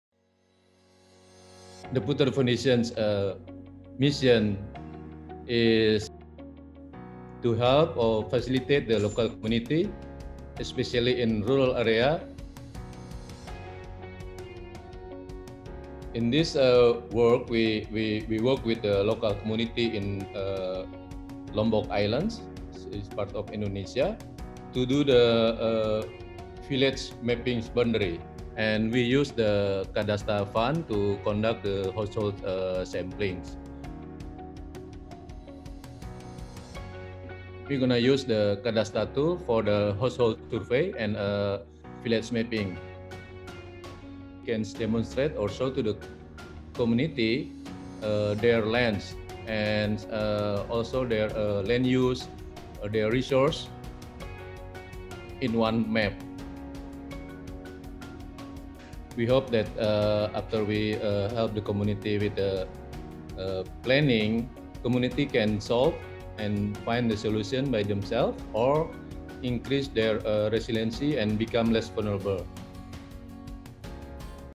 Talking with Cadasta Grantees: Yayasan Puter Indonesia
Learn about the awesome work of our partners and grantees at Yayasan Puter Indonesia!